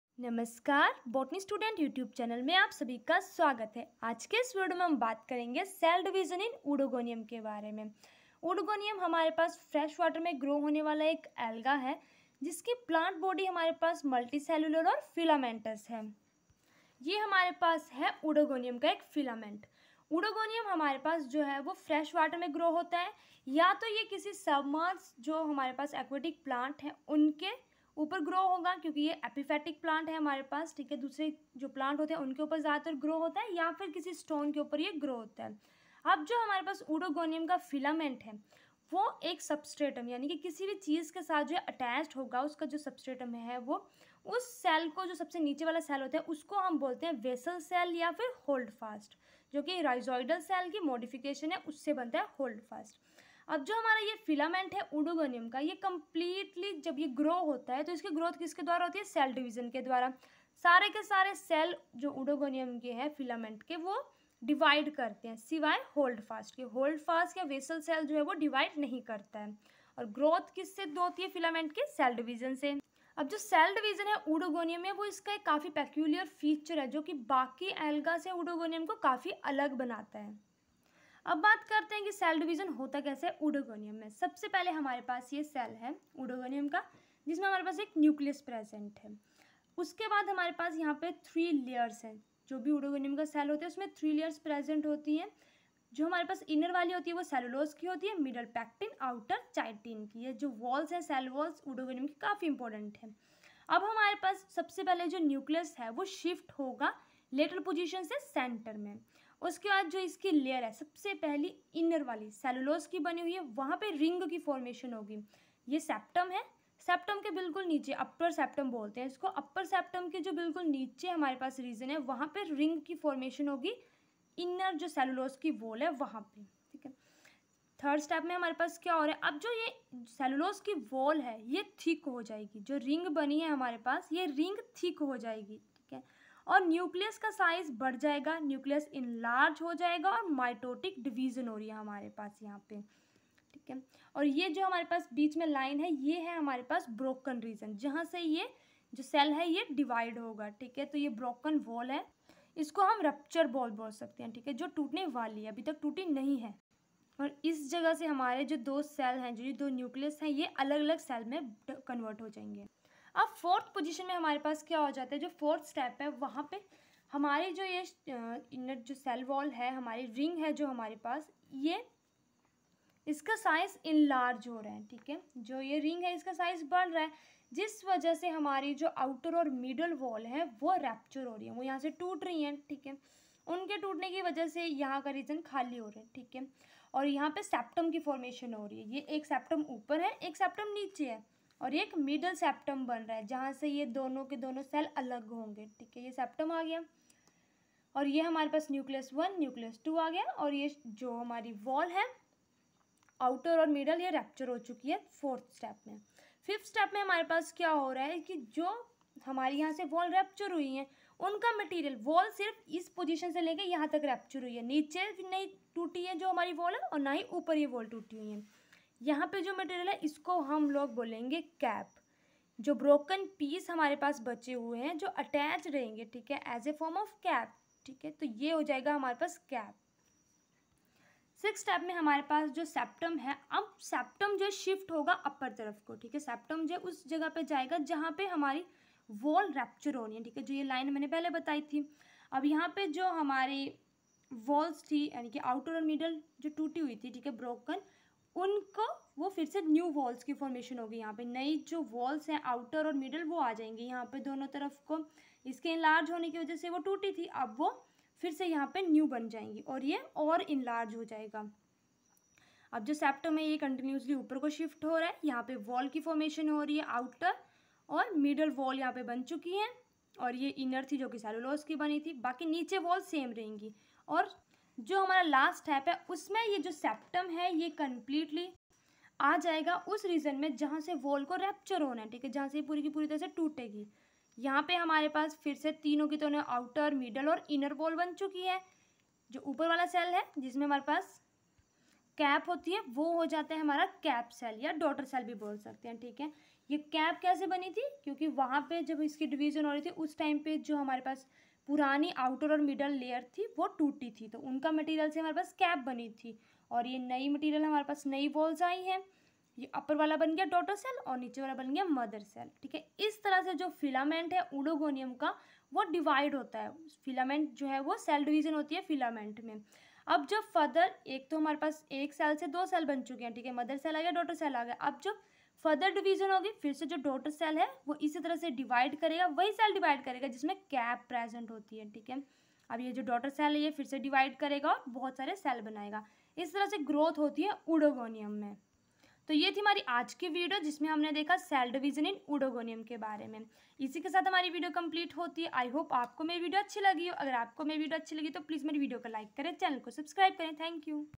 Cell Division in Oedogonium,Oedogonium cell division in detail 🍀☘️🍀☘️
Cell Division in Oedogonium

Pdf of all notes are also available on  our telegram channel